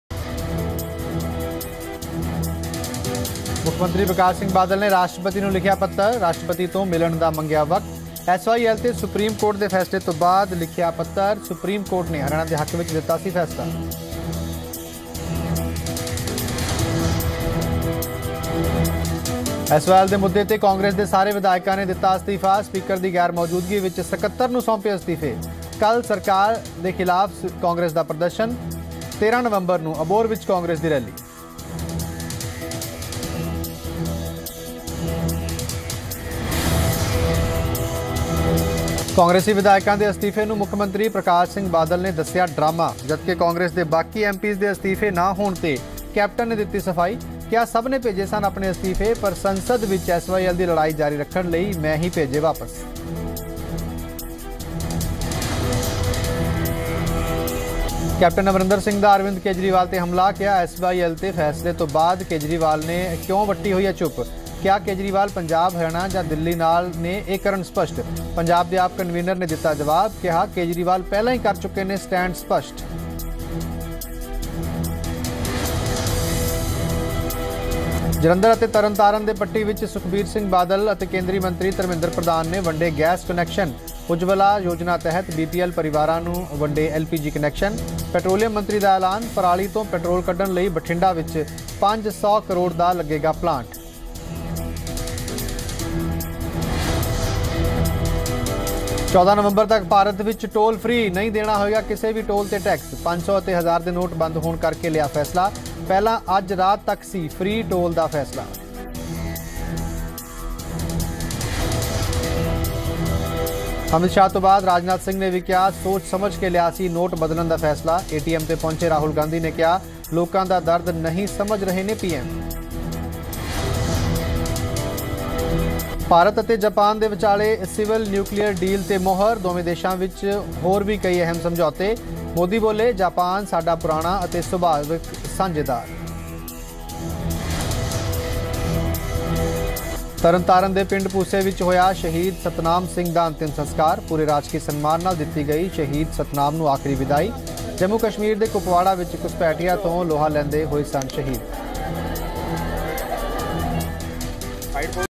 Prakash Singh Badal wrote letter to President for a meeting
Prakash Singh Badal wrote letter to President for a meeting. Watch this special segment and get to know more here.